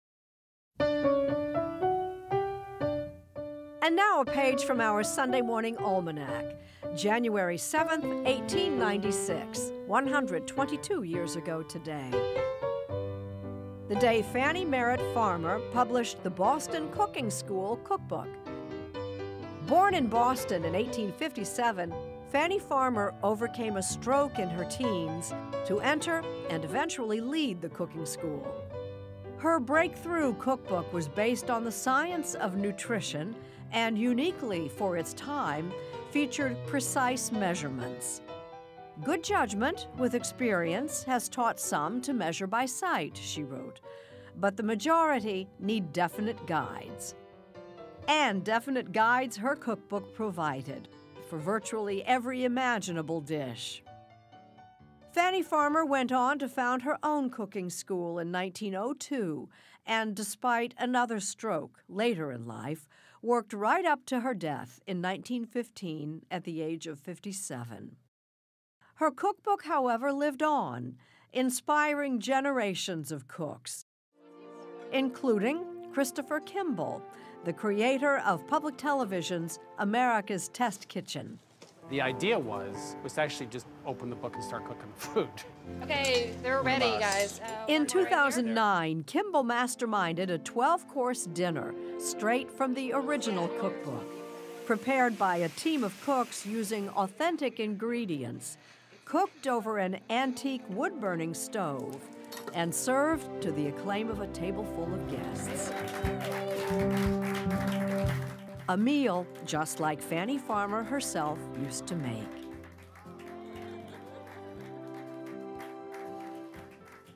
Almanac: Fannie Farmer
On January 7, 1896, the cooking teacher's "Boston Cooking-School Cook Book" was published, inspiring generations of cooks. Jane Pauley reports.